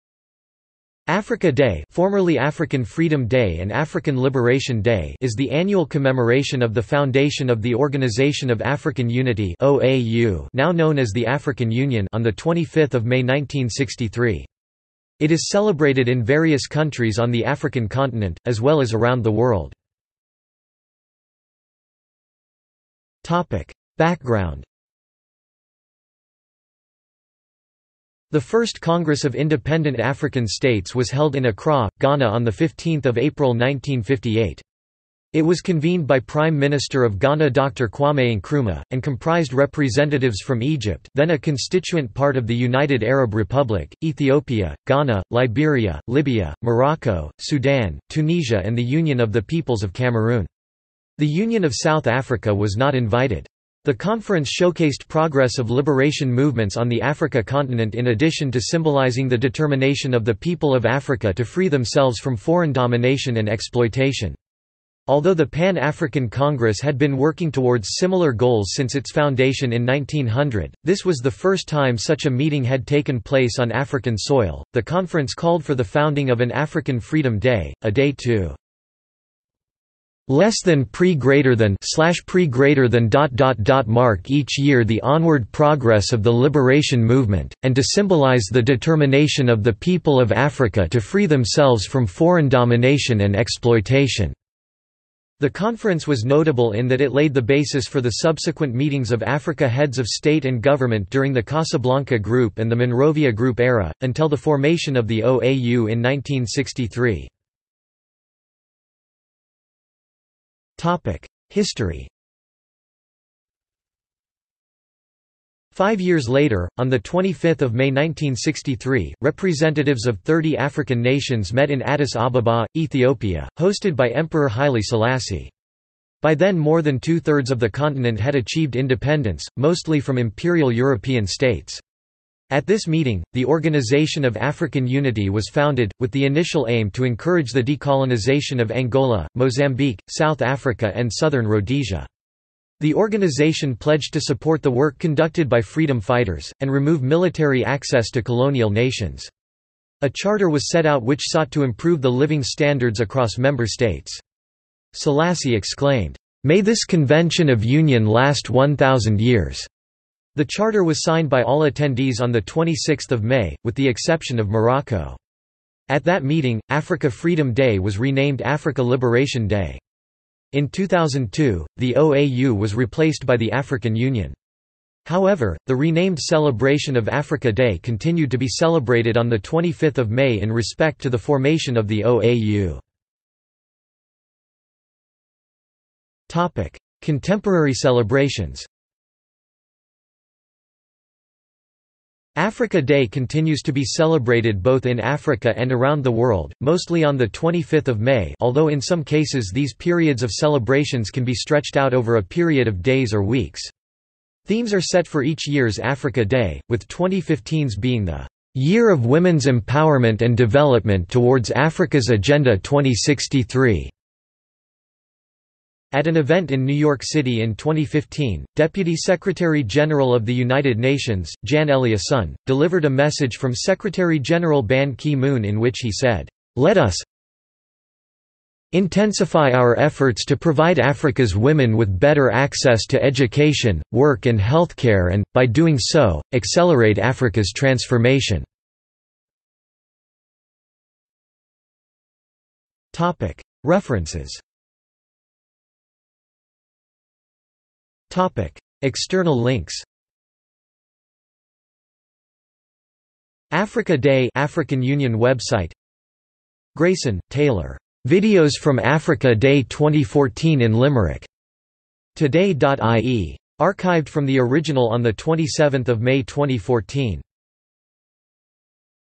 Africa Day | Wikipedia audio article
This is an audio version of the Wikipedia Article:
Africa Day

- Socrates

SUMMARY
Africa Day (formerly African Freedom Day and African Liberation Day) is the annual commemoration of the foundation of the Organisation of African Unity (OAU) (now known as the African Union) on 25 May 1963. It is celebrated in various countries on the African continent, as well as around the world.